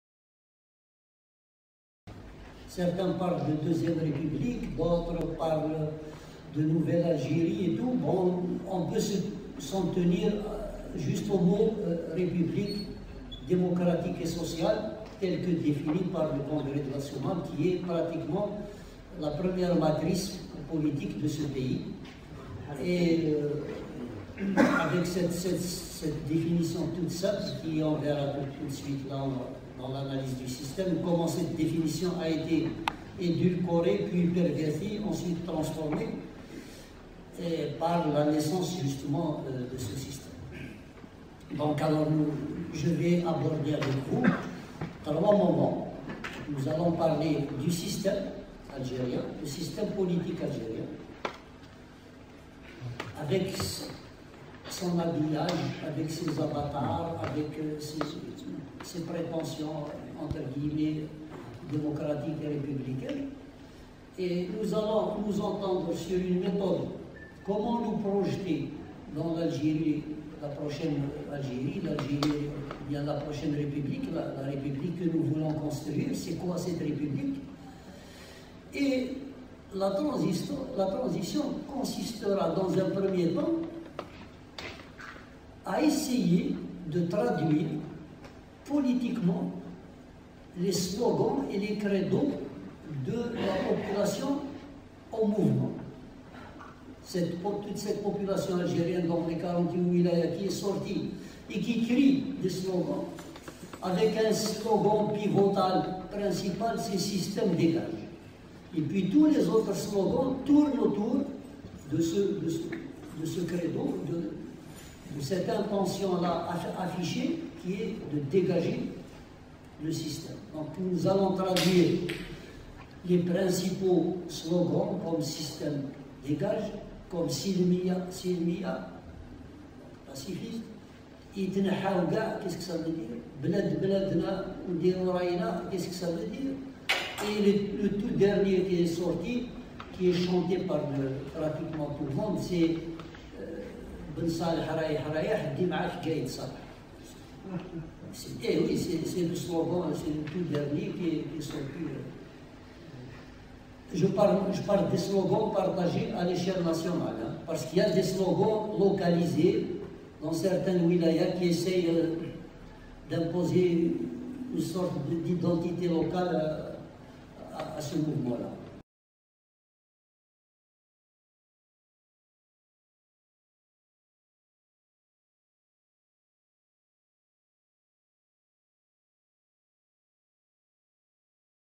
Rachid OULEBSIR Préambule sur ' Quelle feuille de route pour une transition démocratique"
Rachid OULEBSIR : Préambule sur sa conférence ' Quelle feuille de route pour une transition démocratique"
au café littéraire d'Aokas du samedi 18 Mai 2019.

Attendez vous a l'intégralité de la conférence très bientôt